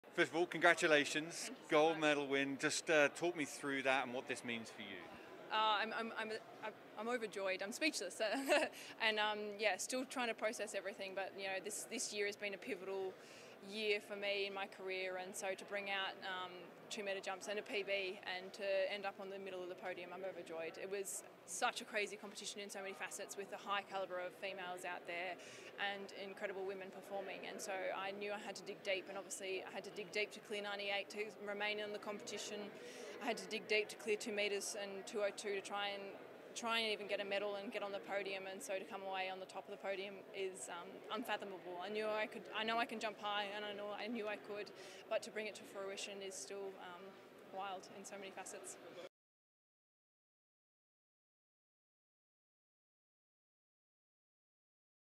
2022 World Athletics Championships: Australia's Eleanor Patterson wins women's high jump
In the women's high jump, Eleanor Patterson cleared an Oceania record of 2.02 meters and become the 10th Australian to win world championships gold. CGTN's Dan Williams spoke with her in Eugene, Oregon.